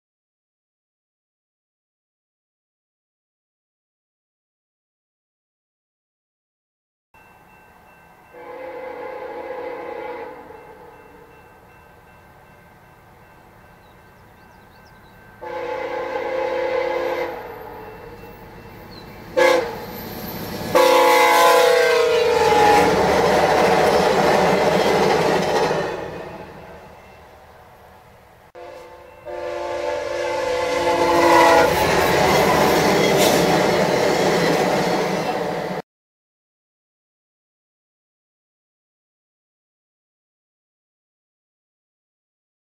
VIA #911 & VIA #919 / 150 ANNIVERSARY UNITS
VIA'S 911EAST & VIA #919 WEST AT MILE 296.22 KINGSTON SUB: (07/20/2017)